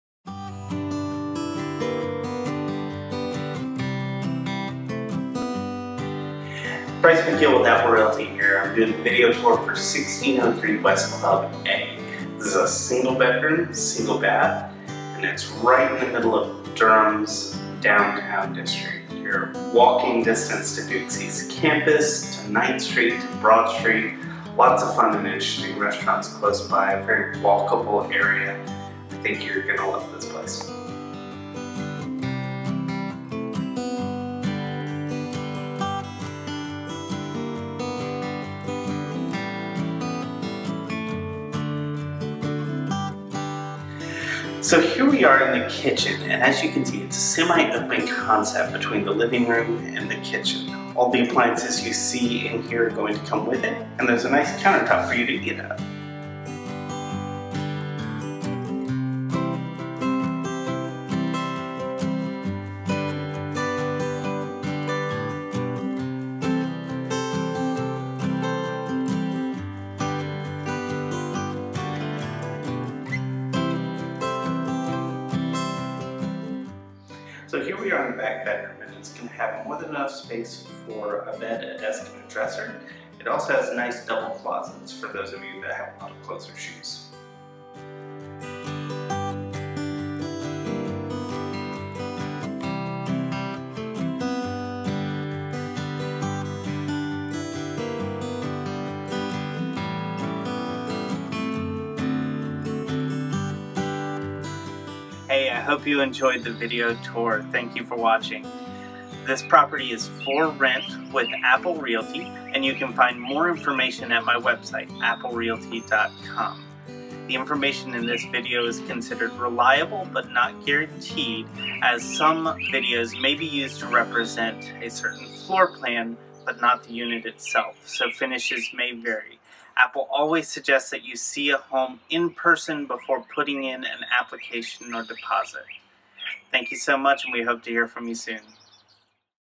1603 West Club Boulevard Apartment A Video Tour
This home is for rent and managed by Apple Realty, a property management company in Durham, NC.  Find further details or other homes to rent Information in the video is considered reliable, but not guaranteed. While we are working on getting videos of every unit, please understand video may be of a home with a similar floor plan.

1BR, 1BA duplex, hardwood floors, kitchen with range and fridge, stacked W/D, gas heat and central A/C